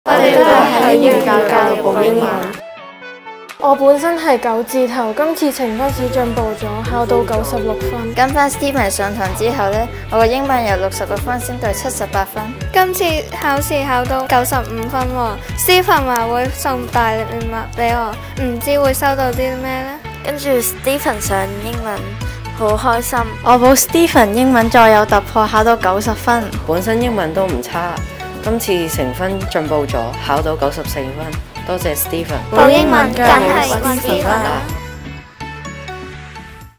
🔥歷時3️⃣0️⃣日🔥Ｐ5️⃣呈分🎯🏫聖公會主愛小學嘅同學跟住Stephen上英文，好開心🥳🎉大家都會得到Stephen嘅獎品🎁
🔥歷時3️⃣0️⃣日🔥Ｐ5️⃣呈分🎯
🏫聖公會主愛小學
👧🏻黃芷晴
今次進步，考到9️⃣6️⃣分！
👧🏻譚茵名: 9️⃣5️⃣分~
Stephen話會送大禮俾我🩷
👦🏻李彥霖: 9️⃣4️⃣分~
多謝Stephen！
👧🏻邱靖兒: 補Stephen~
英文再突破考9️⃣0️⃣分
👦🏻何俊良: 跟返Stephen~
由6️⃣6️⃣分升到7️⃣8️⃣
👧🏻石曉桐:
跟住Stephen上英文，好開心🥳

🎉大家都會得到Stephen嘅獎品🎁
🔥補英文 · 補LAE Stephen
🎯【LAEStephen升🀄呈分特訓🔥】
即刻Inbox我地了解課程資訊啦！！